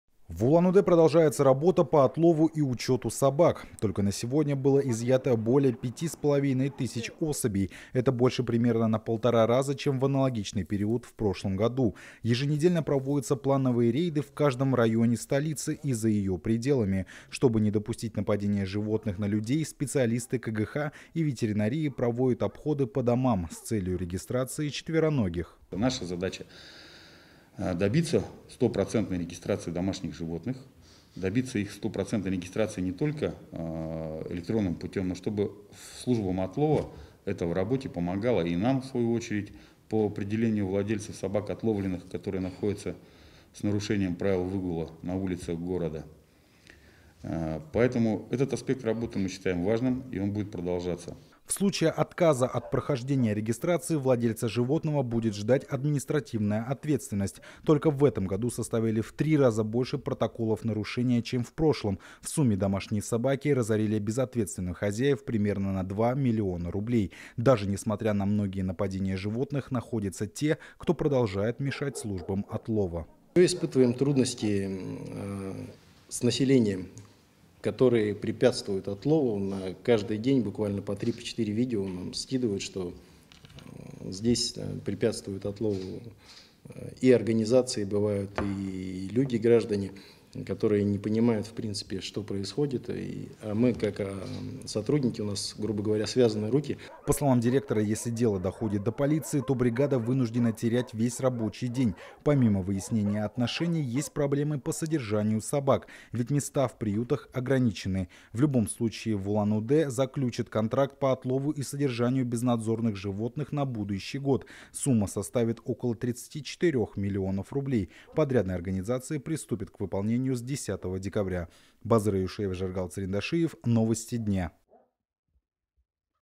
В Улан-Удэ продолжается работа по отлову и учету собак
Только на сегодня было изъято более 5,5 тыс. особей. Еженедельно проводятся плановые рейды в каждом районе столицы и за ее пределами.